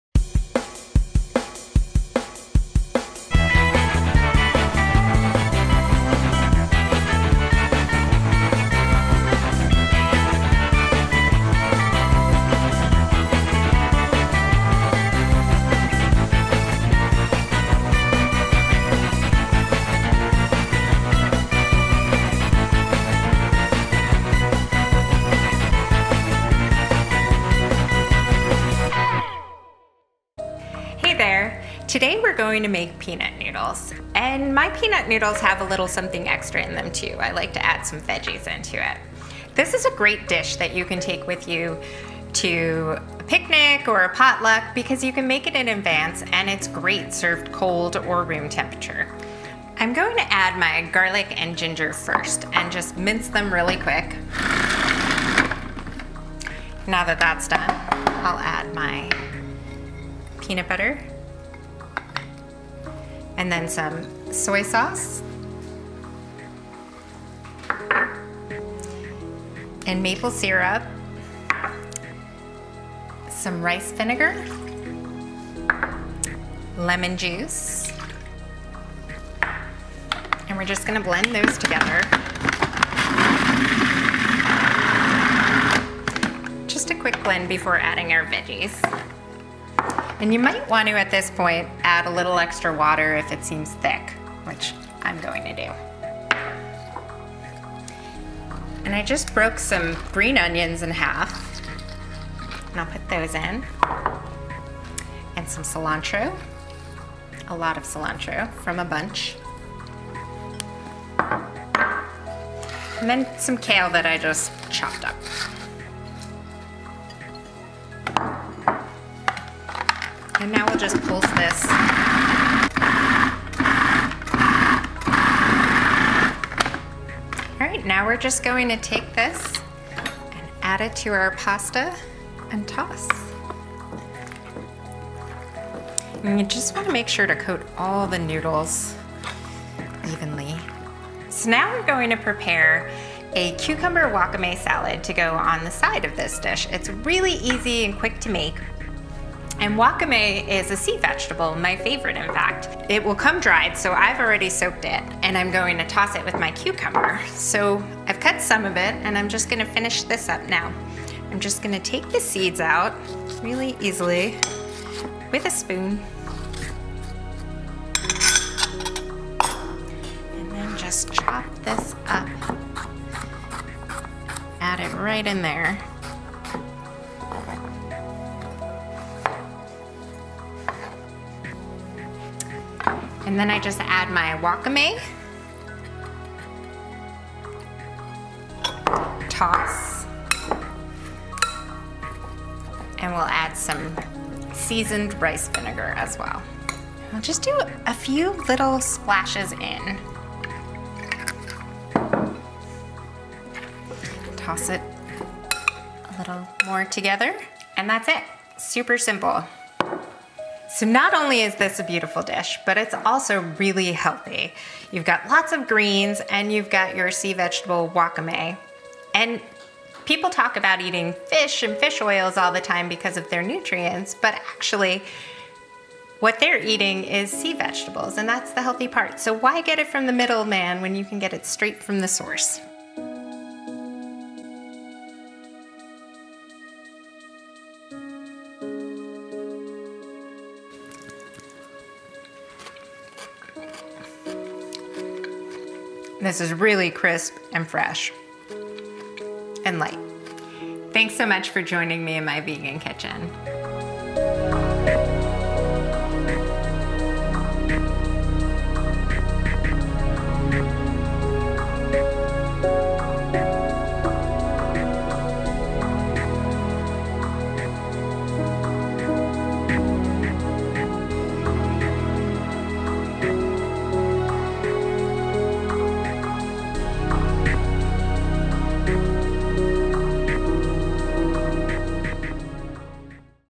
Vegan Peanut Noodles served with Cucumber Wakame Salad recipe
This Peanut Noodle served with Cucumber Wakame Salad recipe is one of the crew's favorites. An Asian-inspired dish that everyone in the family will enjoy. Loaded with flavor and protein, this vegan dish serves as as side or a main entree.

Peanut Sauce
1 C Peanut Butter
1/4 C maple syrup
2 T soy sauce
1/4 C vinegar
1 inch fresh ginger
1 clove garlic
1/4 C lemon juice
1 bunch Green Onions
1/4 C cilantro leaves and 5 kale leaves pulsed at the end
1 lb. Rice noodles, spaghetti, or udon noodles

Blend sauce ingredients until smooth. Add Cilantro, Kale, and Green onions and pulse until chopped. Stir into cooked noodles.

Cucumber Wakame Salad:
3 Cucumbers
1/3 C wakame seaweed
2T Seasoned Rice Vinegar

Soak wakame in cool water. Peel and Seed cucumbers. Cut in half lengthwise and thinly slice each half and place in bowl. Add wakame and rice vinegar and toss. Serve over/on side of noodles.